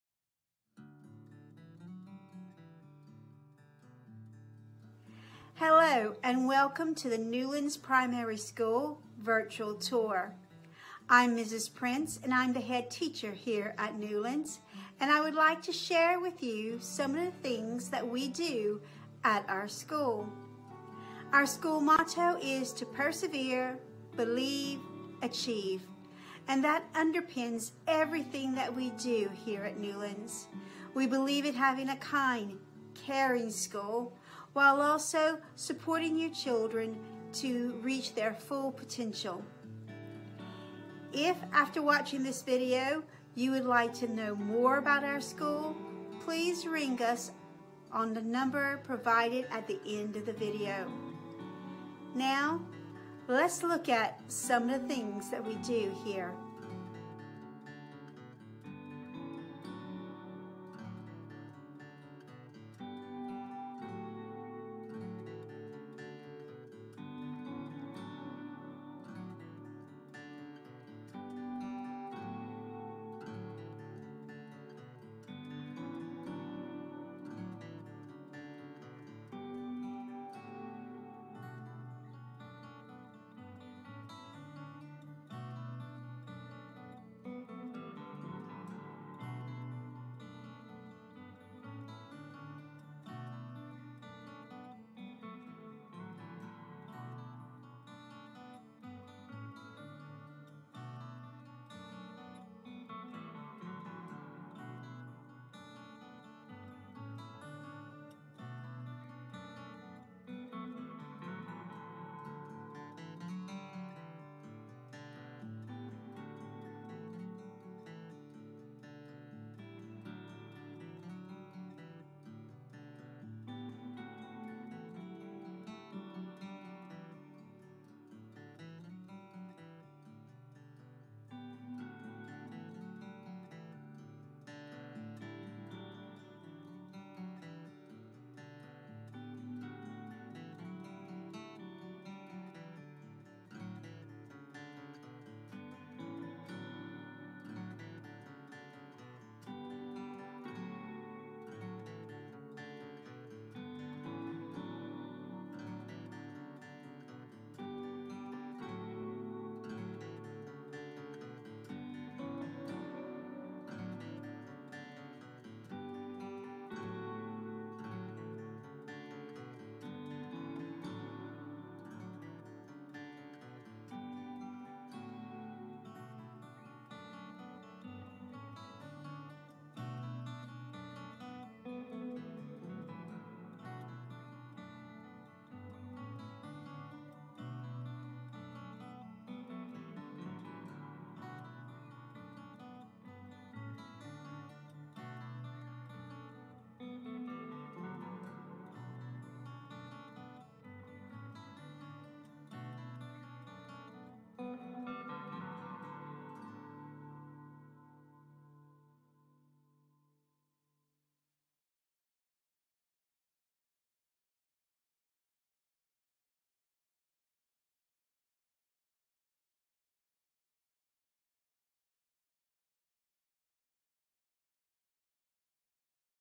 Newlands Primary Virtual School Tour 2021-2022
Welcome to the Newlands Primary School Virtual Tour.
We invite you to watch our video and learn a bit more of our school and what we have to offer. We also encourage you to contact school on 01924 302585 for more information.